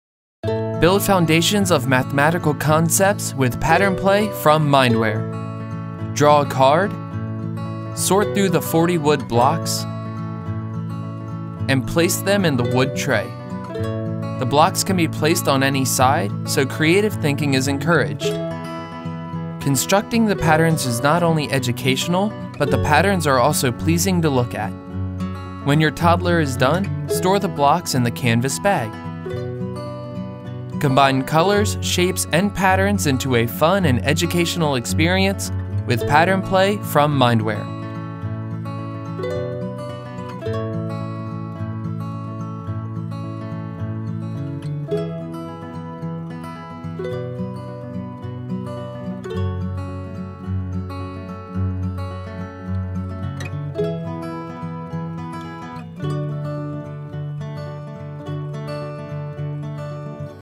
Mindware - Pattern Play Nasıl Oynanır?
Çok yönlü oynanabilen Desen Oyunu sıralama, eşleştirme, uyum ve kesirler gibi matematik kavramları anlamaya yönelik yapılan bir oyundur.